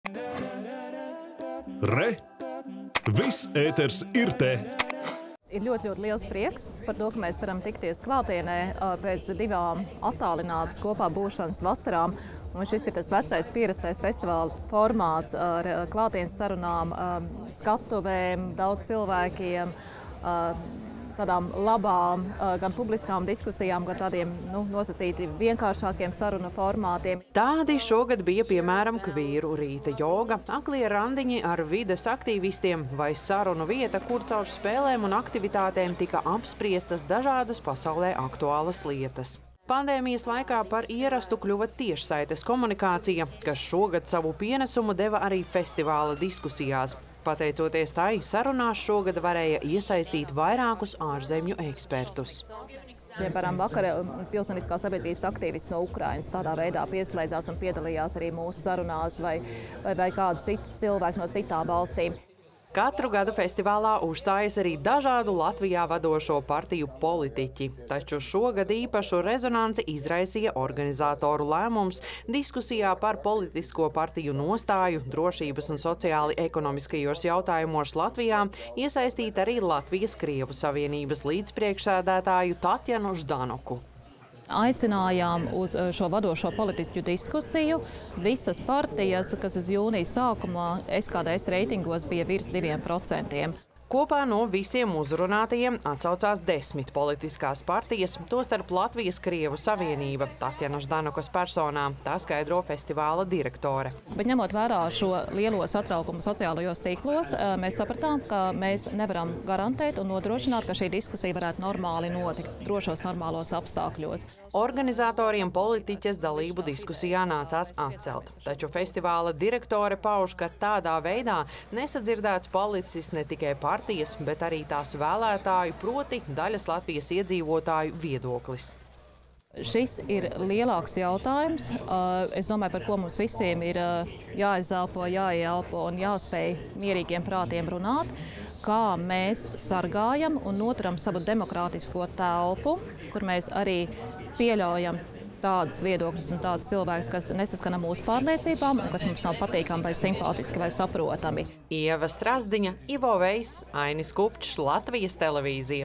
Cēsīs aizvadīts ikgadējais festivāls LAMPA
Ar kārtīgu Latvijas politiķu cepināšanu Cēsīs sestdien tika noslēgts ikgadējais sarunu festivāls Lampa. Šogad uz trīsdesmit trīs festivāla skatuvēm tikās gandrīz tūkstoš dalībnieku un runātāju.
    Un organizatoru apkopotā informācija liecina, ka festivālu apmeklēja deviņpadsmit tūkstoši cilvēku. Vairāk stāsta mūsu kolēģe Ieva Strazdiņa.